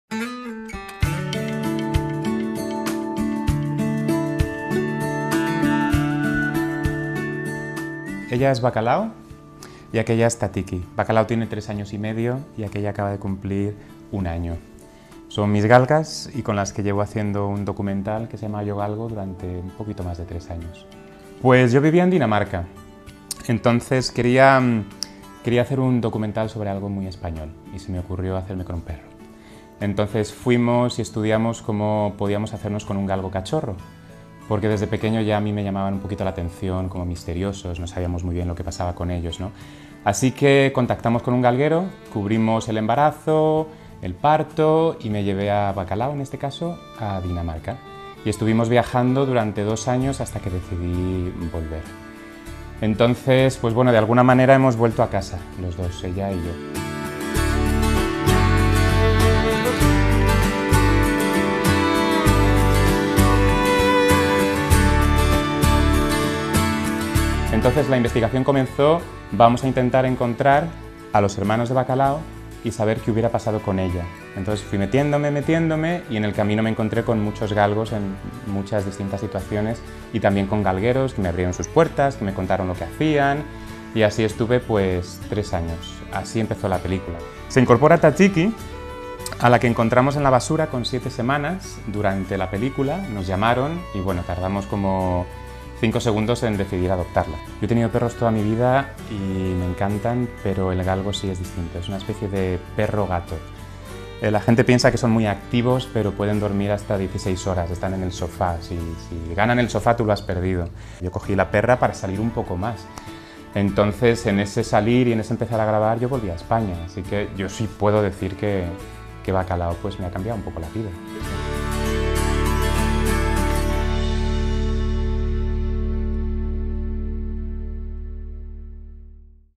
Yeray López, salsa de ‘Bacalao’ y ‘Tzatziki’ | Amos y mascotas | El País Semanal
El diseñador gráfico Yeray López atravesaba una depresión. Pensó que un perro le ayudaría y contactó con un galguero en España. Escogió esa raza para conocer a fondo su realidad y hacer un documental. De los cinco cachorros que le ofrecieron, se quedó con una: Bacalao.